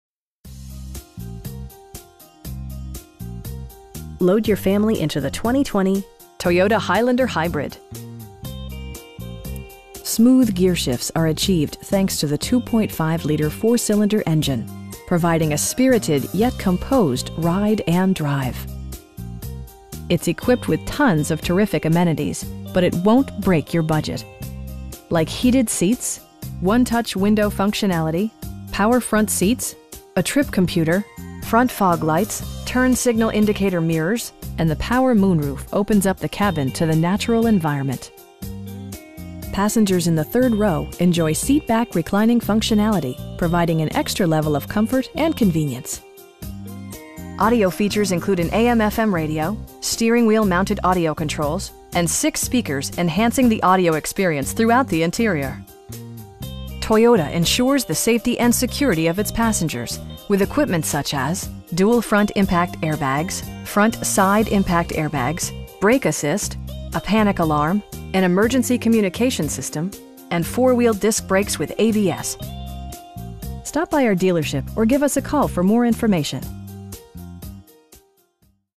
2020 Toyota Highlander Hybrid XLE in Vienna, VA 22182
Load your family into the 2020 Toyota Highlander Hybrid. Smooth gearshifts are achieved thanks to the 2.5 liter 4 cylinder engine, providing a spirited, yet composed ride and drive. It's equipped with tons of terrific amenities, but it won't break your budget. Like heated seats, 1-touch window functionality, power front seats, a trip computer, front fog lights, turn signal indicator mirrors, cruise control, and the power moon roof opens up the cabin to the natural environment. Passengers in the third row enjoy seat back reclining functionality, providing an extra level of comfort and convenience. Audio features include an AM/FM radio, steering wheel mounted audio controls, and 6 speakers, enhancing the audio experience throughout the interior. Toyota ensures the safety and security of its passengers with equipment such as: dual front impact airbags, front side impact airbags, traction control, brake assist, a panic alarm, an emergency communication system, and 4 wheel disc brakes with ABS. With electronic stability control supplementing mechanical systems, you'll maintain precise command of the roadway. We have the vehicle you've been searching for at a price you can afford. Stop by our dealership or give us a call for more information.